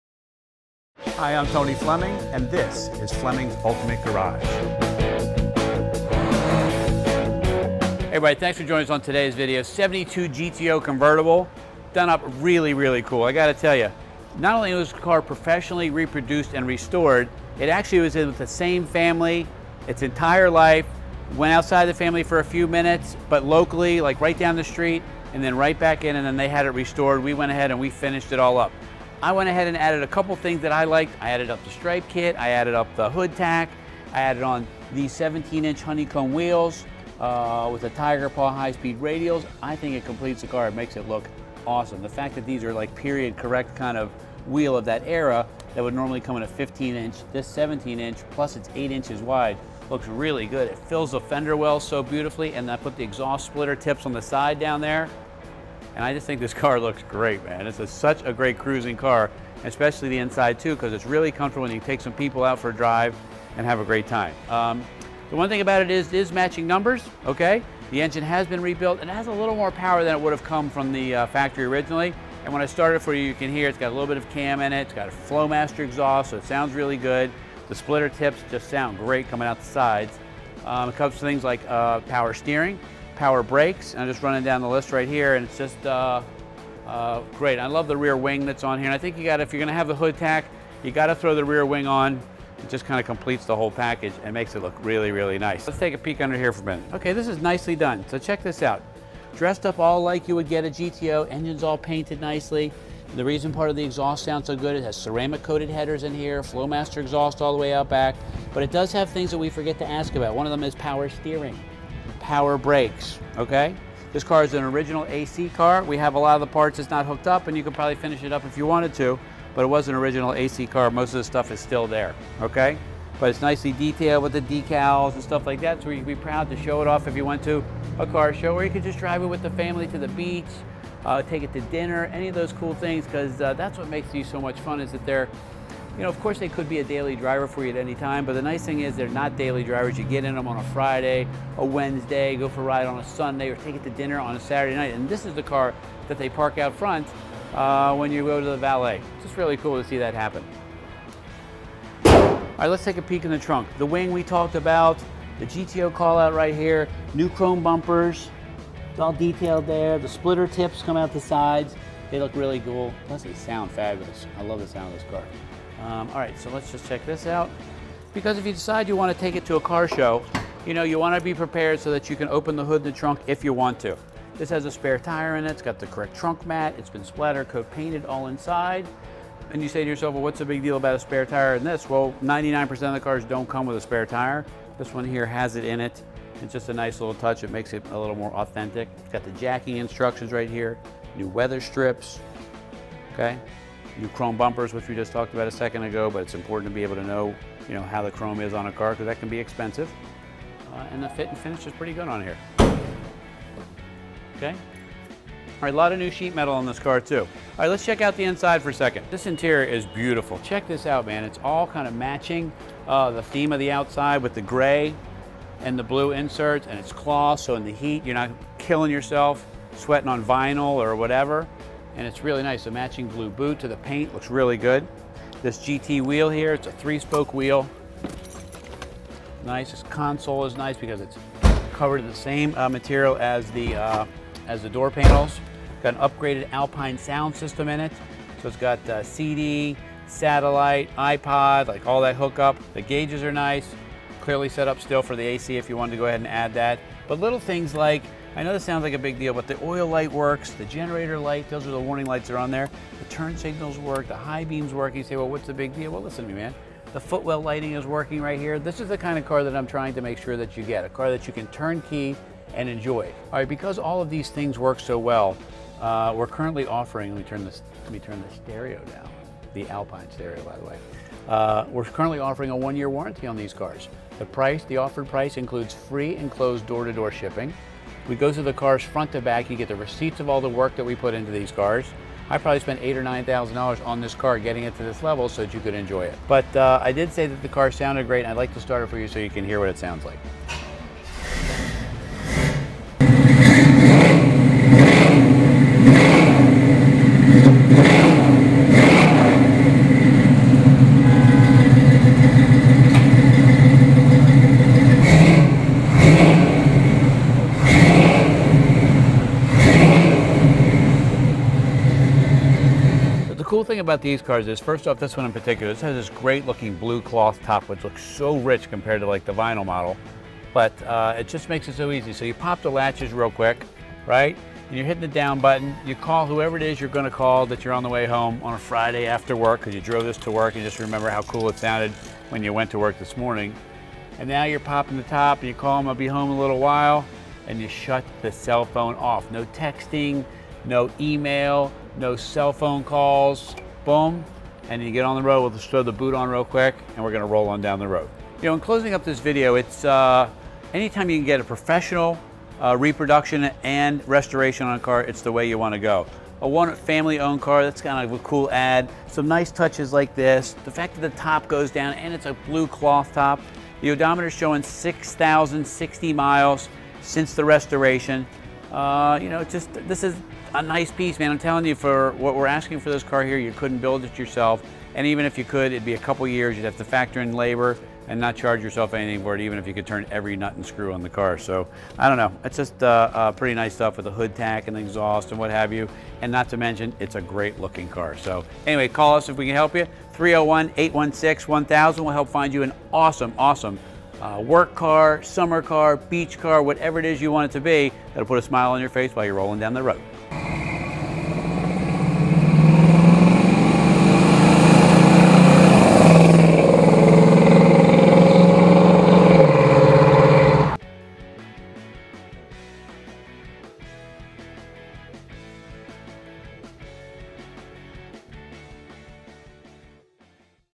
1972 Pontiac GTO Convertible for sale with test drive, driving sounds, and walk through video
Recently restored and appraised, rebuilt matching #'s 4bbl V-8, rebuilt automatic transmission, great looking medium blue metallic paint, custom grey and blue cloth bucket seat interior with factory console, great looking blue German canvas power convertible top with matching boot, power disc brakes, power steering,17" Pontiac Honeycomb alloys with fresh hi-speed radials, cool factory hood tach, rear wing, great sounding dual exhaust w/ splitter tips, new chrome & stainless, detailed engine compartment, detailed trunk, upgraded digital sound system, nicely restored, professionally reproduced and one great driving cool convertible.